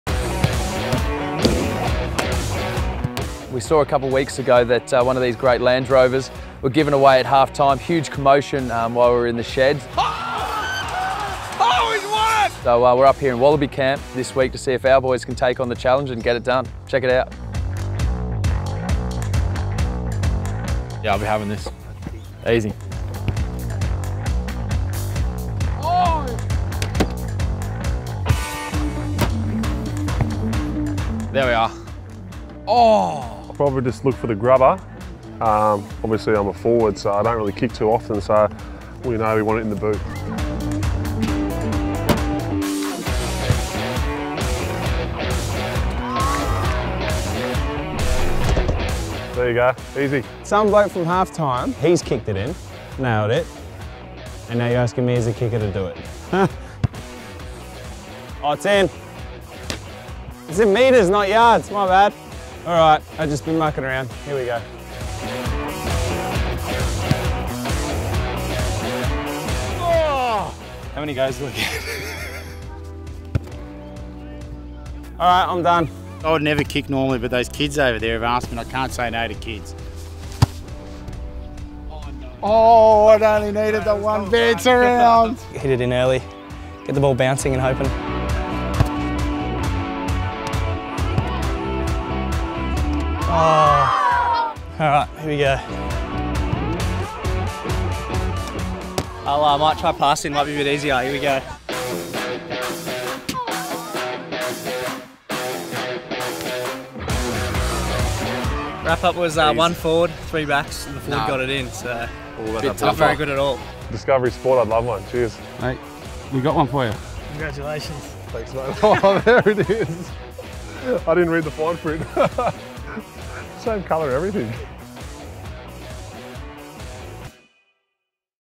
Wallabies take on the Land Rover Kick to Win Challenge | Land Rover Rugby
After seeing a lucky Wallabies fan conquer our Kick to Win challenge and win a Discovery Sport, we challenged the Wallabies to do the same. Watch to see how they fared.